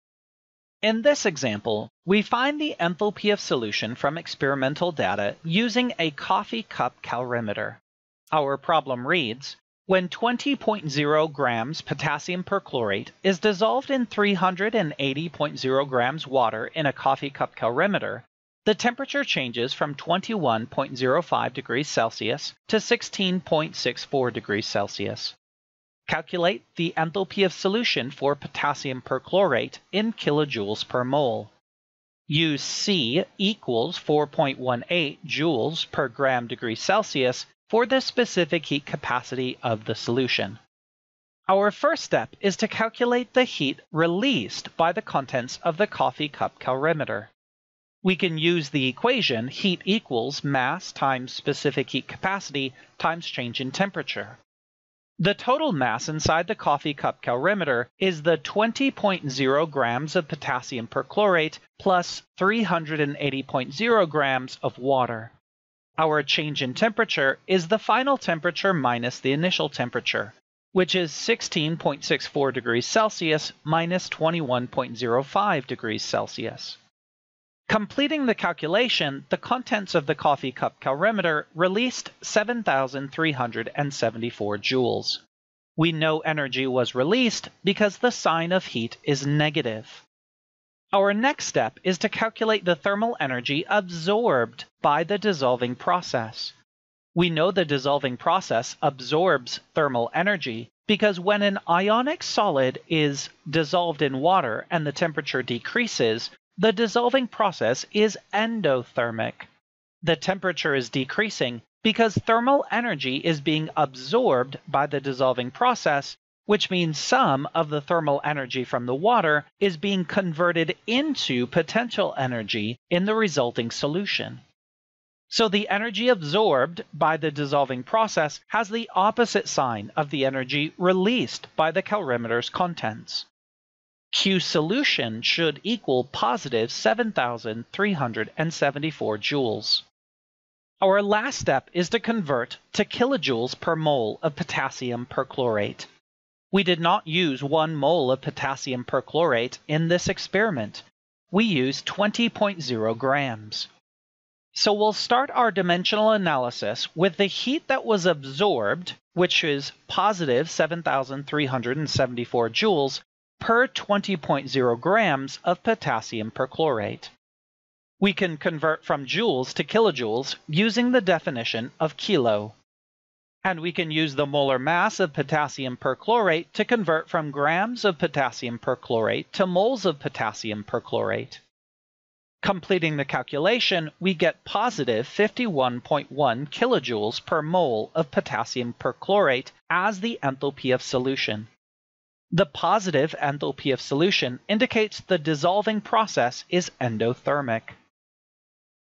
CHEM 101 - Calculating Enthalpy of Solution 2
In this example, we find the enthalpy of solution from experimental data using a coffee-cup calorimeter.

When 20.0 g potassium perchlorate is dissolved in 380.0 g water in a coffee-cup calorimeter, the temperature changes from 21.05 °C to 16.64 °C.  Calculate ΔHsolution for potassium perchlorate in kJ/mol. Use c = 4.18 J/(g °C) for the specific heat capacity of the solution.